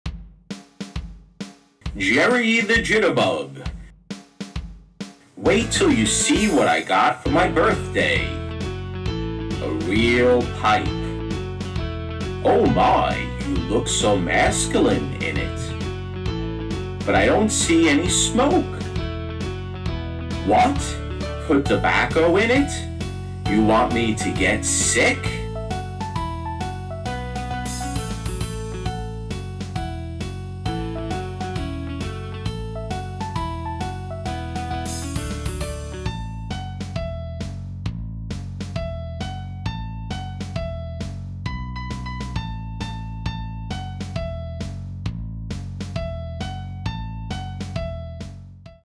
Jerry the Jitterbug 1
Here's an old strip from an old comic with voices kindly provided by the @jerrythejitterbugcomicbook4098 channel. Check him out for awesome comic restoration videos!

Kevin Hole
Colin Tyler
Elric Newby (Into the Pit)
Federico Pierandrei
Linz